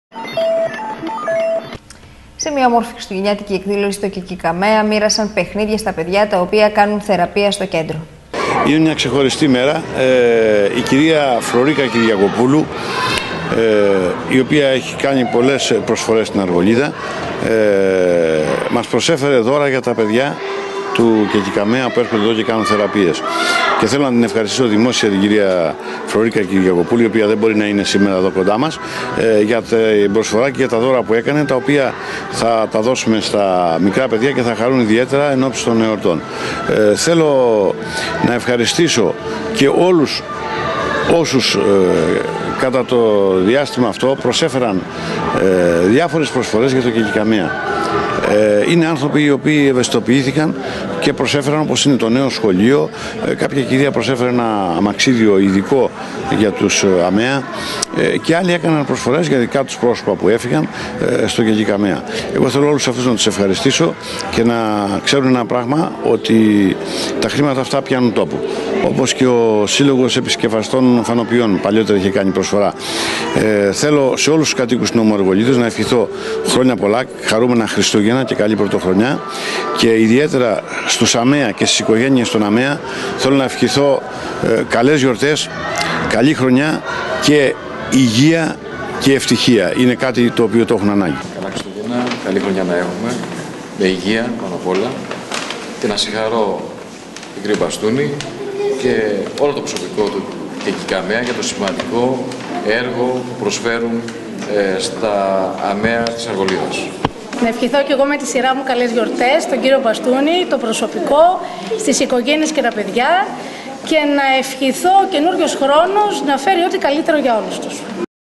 ΑΡΓΟΛΙΔΑ 24-12-2009 DRTV
Αργολίδα - Μοίρασαν δώρα στα παιδιά στο ΚΕΚΥΚΑΜΕΑ.